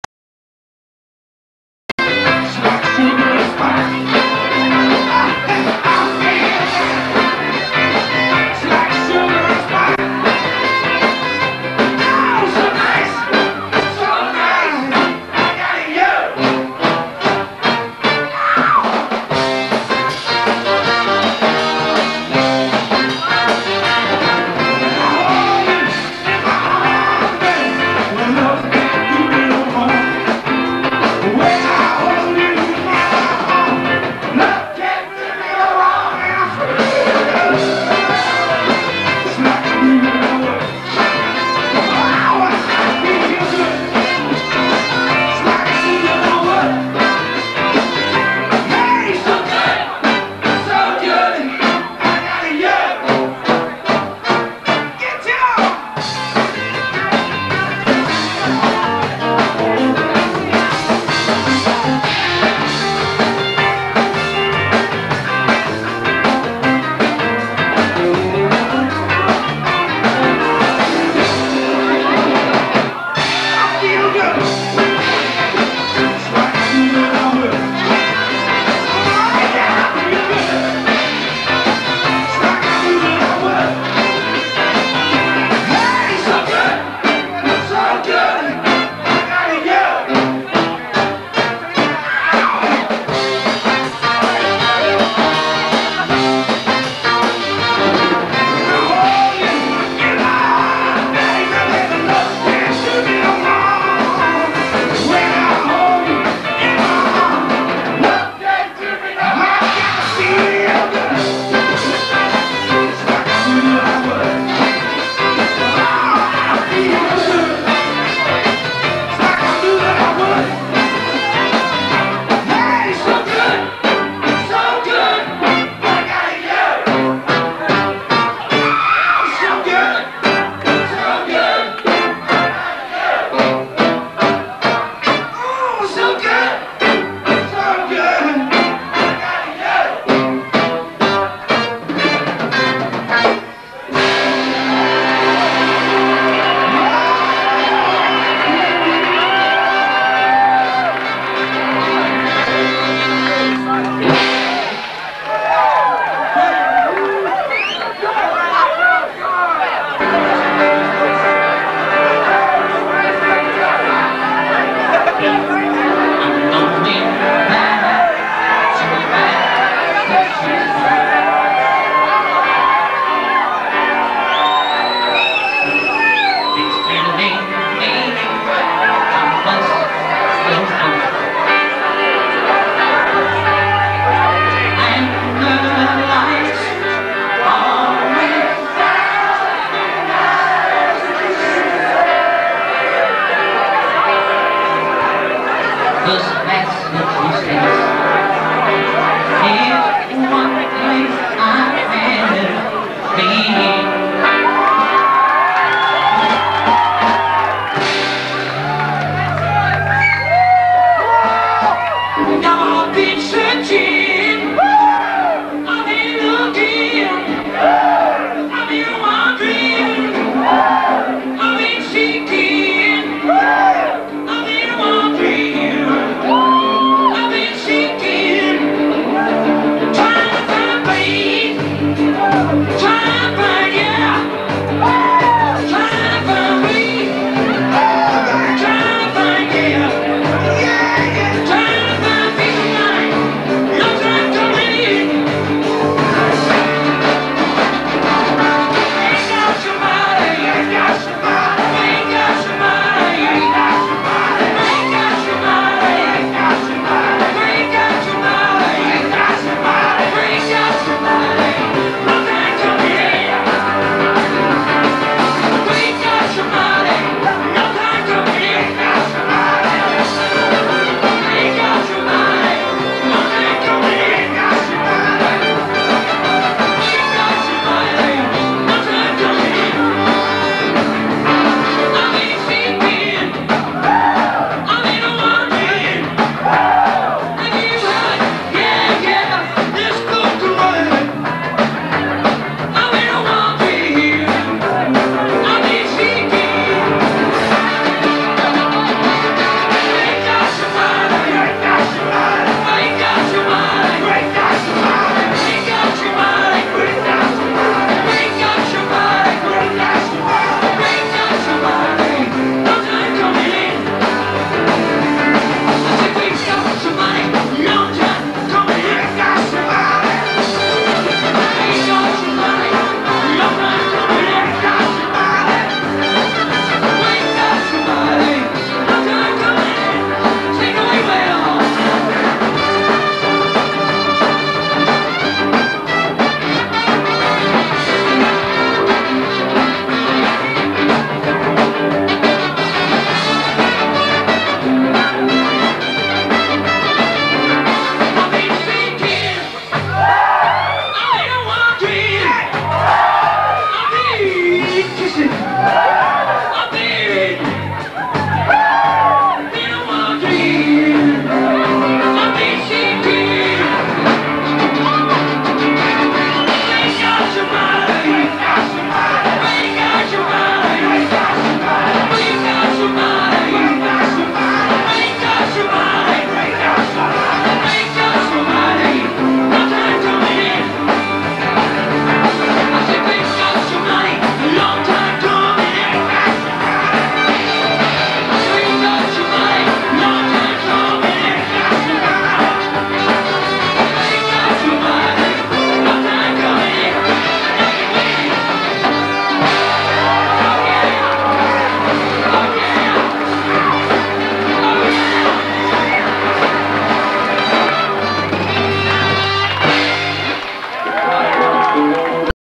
Big 5 live medley - 1991
big5 - ye olde leather bottle 30 years ago!!! billy franks, lee hirons, steve howlett, tony carpenter, Will tipper - medley - bits great trumpet!